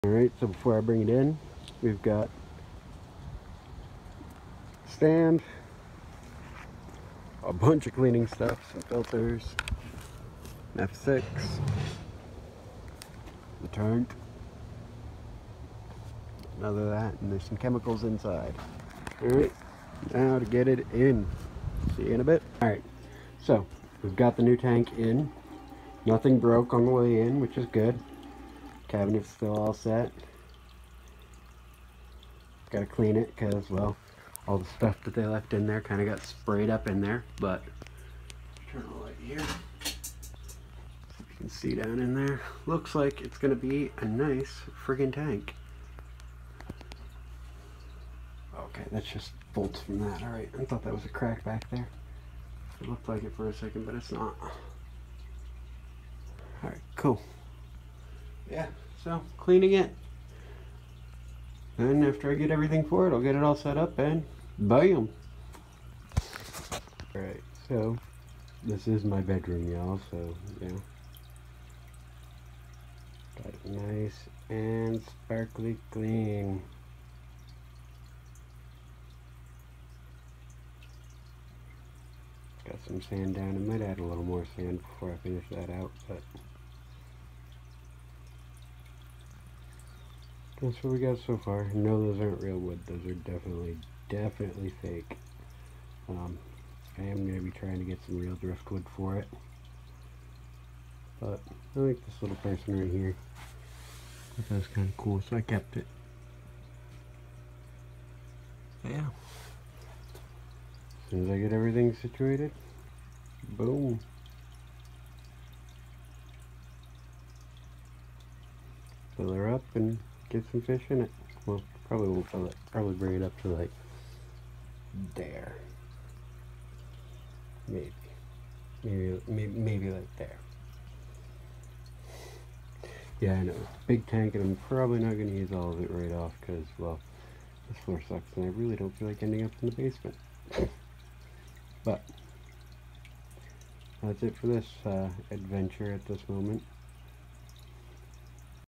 The 150 gallons arrival. Cleaning. And original thoughts on set up
Current tank : 29 gal fb marketplace find
Current filter : the one that came with it a 2” aquarium solutions bacto-surge tm sponge filter
Current heater : 1 100w Aqueon heater non adjustable 1 200w heater adjustable - set temp - 80*F
Current substrate : pool filter sand
Current stars : 1 male Betta Splendid - type - multi color hmpk - name - Joe Carr.
2 female Betta Splendid - type - blue hmct, marble blue hmct - names - Caitlin Snow, Princess.
1 Hypostomus plecostomus - type - brown common - name - Plucky.
1 Ancistrus - type - Ancistrus leucostictus - name - Houdini.
3 Corydoras paleatus - type -  peppered Cory Cat - names - Peppa, Spot, Un-Named.
3 Corydoras aeneus - type - albino Cory Cat - names - Ghost, Whitey, Buddy.
1 Epalzeorhynchos bicolor - type - red tailed black shark - name - Lucky.
Planorbidae - type - white brown common ramshorn snail - names - The Ramas Horns.
9 Pomacea bridgesii - type - light chestnut, dark chestnut, 3x blue, 2x ivory 2x golden Mystery Snails - names - Pokey, Speedy, Blue, unnamed, unnamed, I’ve ree, sssslllloooowwww, unnamed, unnamed.
4 Palaemonetes paludosus - type - Ghost Shrimp - names - Pinky, Blinky, Winky, Clyde.
5  Caridina multidentata - type - Amano Shrimp - names - The Shrimp
Melanoides tuberculata - type - common brown Malaysian trumpet snail - names - Tha trumpets.
Current plants : several of each no names given to the plants
Hornwort - species - Ceratophyllum demersum
Red Dwarf Lily - species - Nymphaea stellata
Water Wisteria - species - Hygrophila difformis
Crested Java Fern - species - Microsorium pteropus sp.